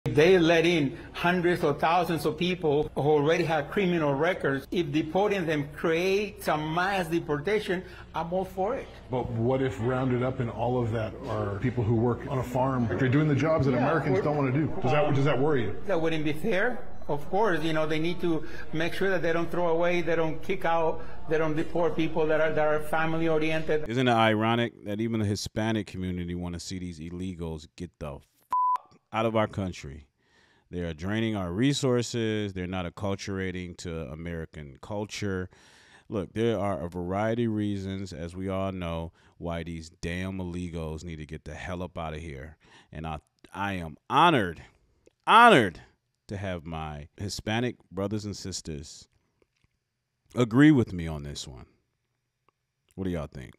Round them up and ship 'em back!
Hispanic man says he's all for mass deportation of illegals. Is he wrong??